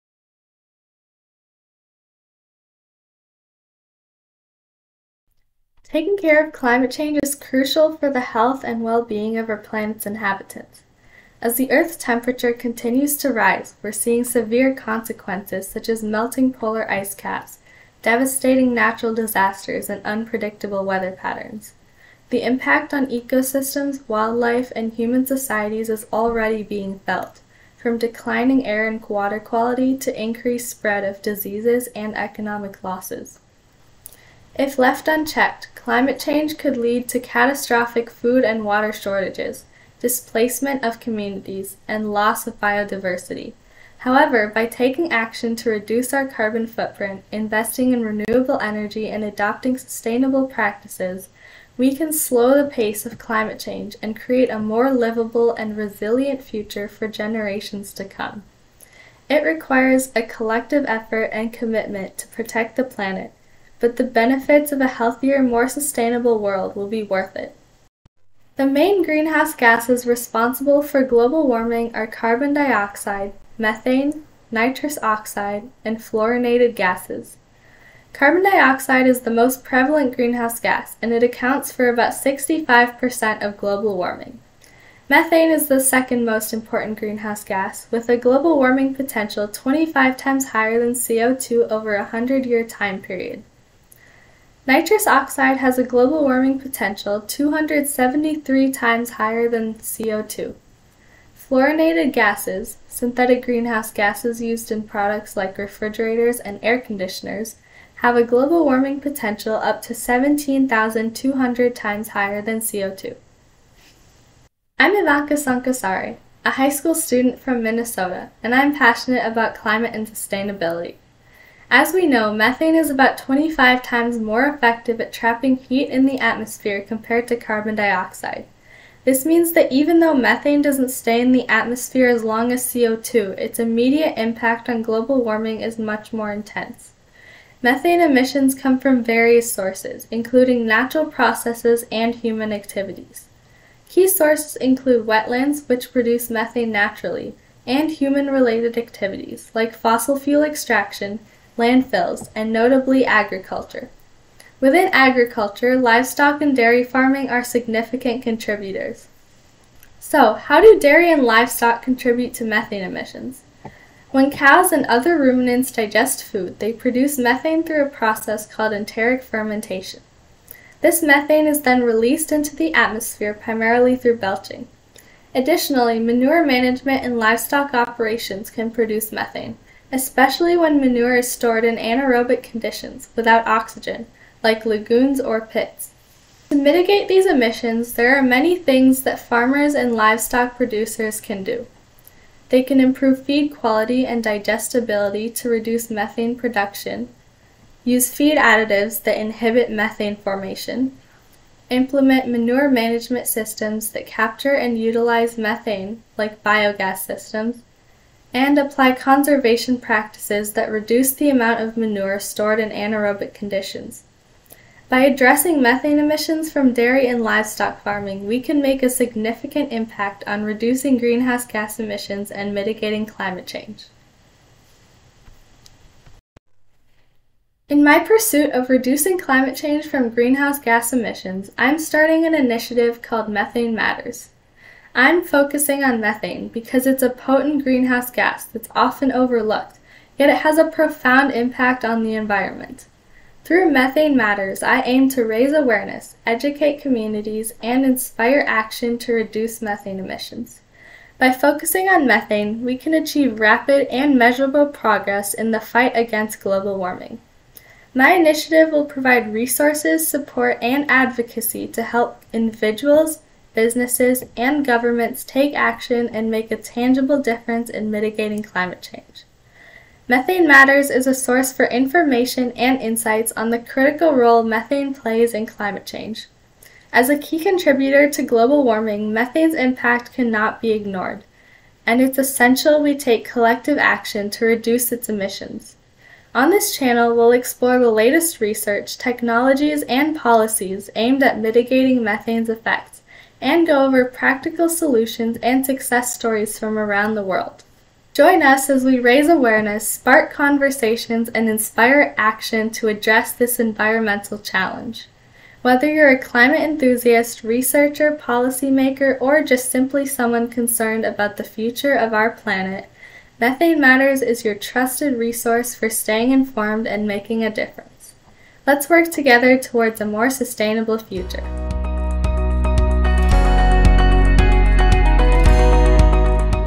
MethaneMatters | Introduction to Global Warming and Why Methane Matters
This video provides a comprehensive introduction to global warming and the significant impact of methane on the environment. Starting with the basics of global warming, the video delves into the role of methane, a potent greenhouse gas, in accelerating climate change. You'll learn about the sources of methane emissions, its effects on the environment, and the devastating consequences of inaction. The video also touches on the importance of methane reduction strategies and the role individuals can play in mitigating its impact. A great starting point for those looking to understand this critical issue.